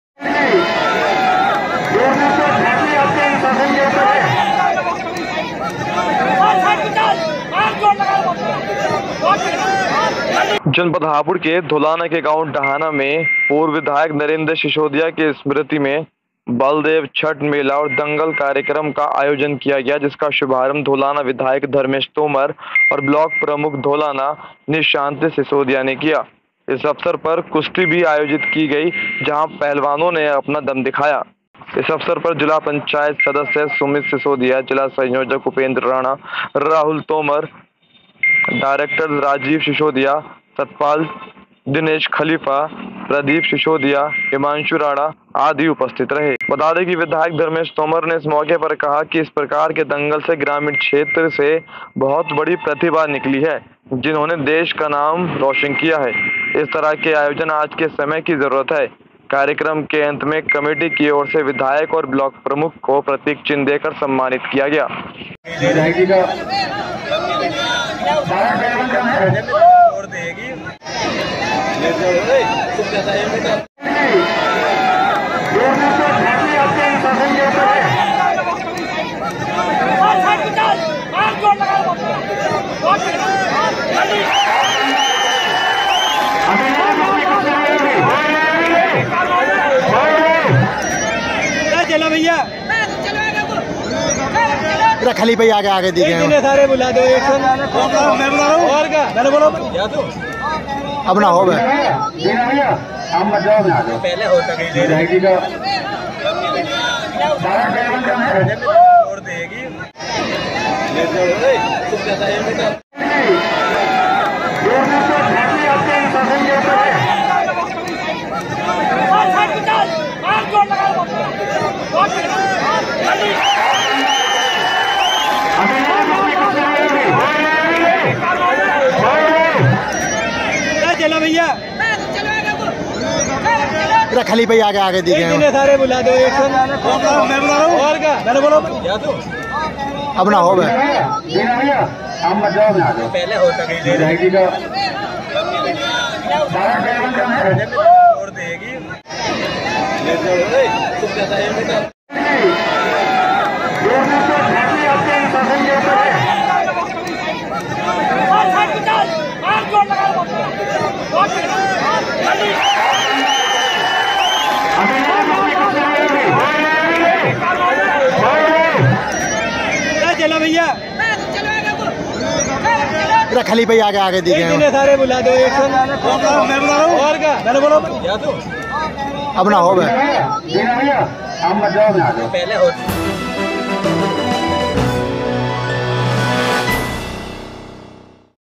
धौलाना में कुश्ती का आयोजन
जनपद हापुड़ के धौलाना के गांव डहाना में पूर्व विधायक नरेंद्र शिशोदिया की स्मृति में बलदेव छठ मेला व दंगल कार्यक्रम का आयोजन किया गया जिसका शुभारंभ धौलाना विधायक धर्मेश तोमर व ब्लॉक प्रमुख धौलाना निशान्त शिशौदिया ने किया। इस अवसर पर कुश्ती भी आयोजित की गई।
विधायक धर्मेश तोमर ने इस मोके कहा की इस प्रकार के दंगल से ग्रामीण क्षेत्र से बहुत बड़ी प्रतिभा निकली है जिन्होंने देश का नाम रोशन किया है। इस तरह के आयोजन आज के समय की जरुरत है। कार्यक्रम के अंत मे कमेटी की तरफ से विधायक व ब्लॉक प्रमुख को प्रतीक चिन्ह देकर सम्मानित किया.